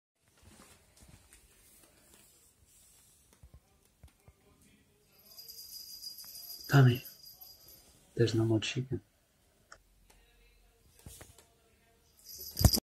Telling my cat: no more chicken. This is what happened next
Tommy gets mad when he finds out there's no more chikan...

Hes doesn't take negative news very well..